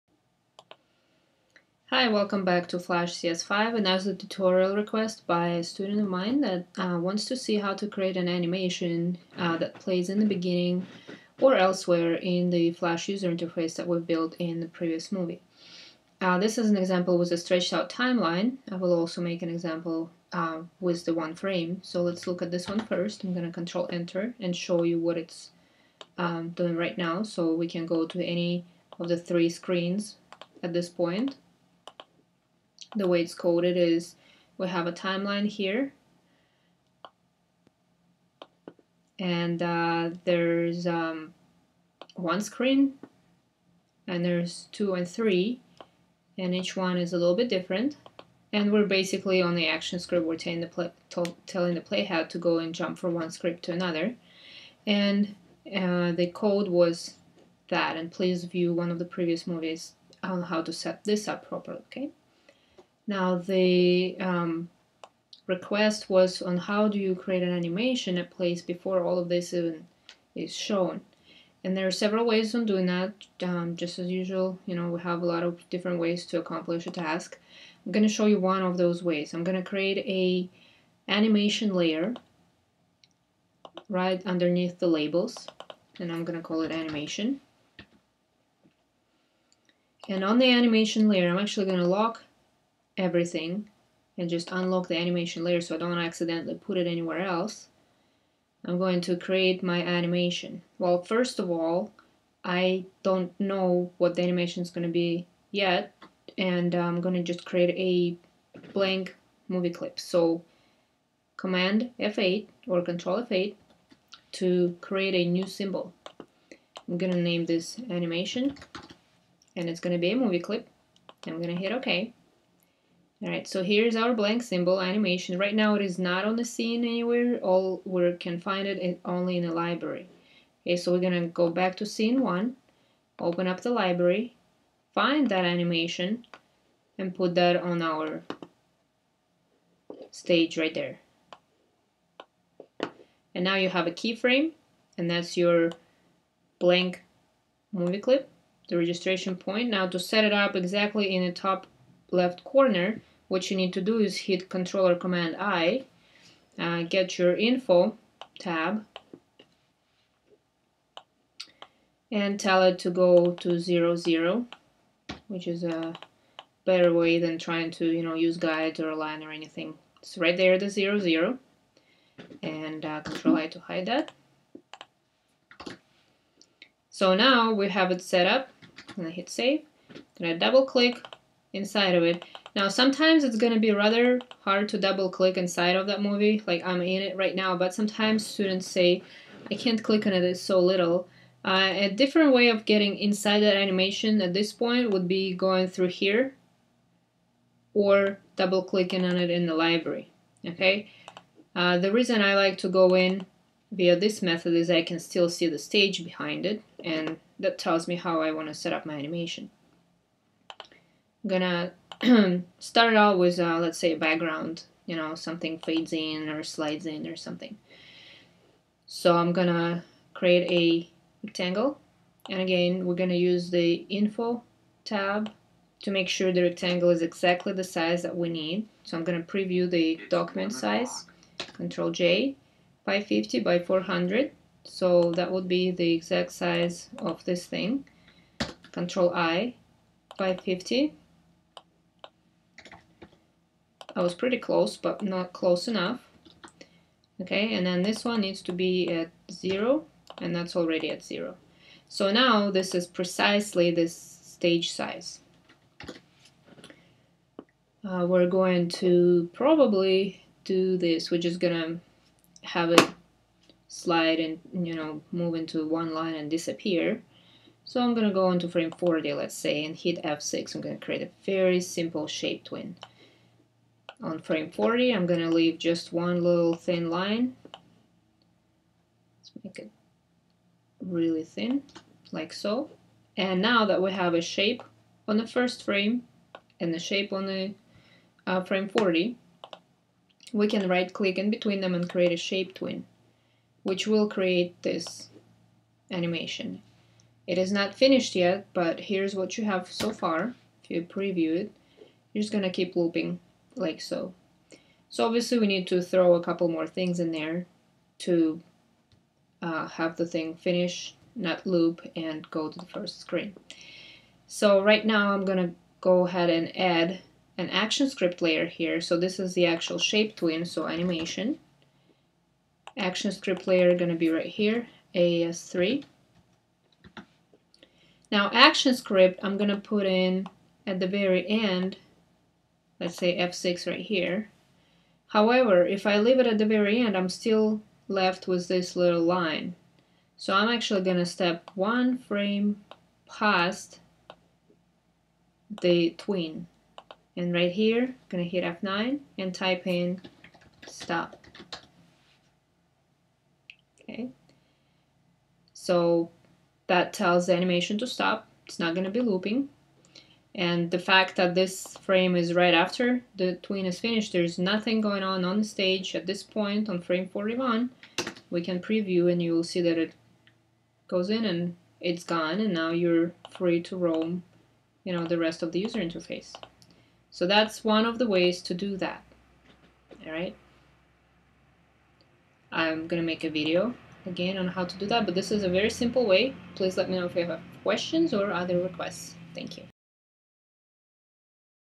AS3-UI_ST-IntroAnim.mov
Flash CS5 Actionscript 3.0 tutorial: setting up a User Interface Intro Animation (stretched timeline UI).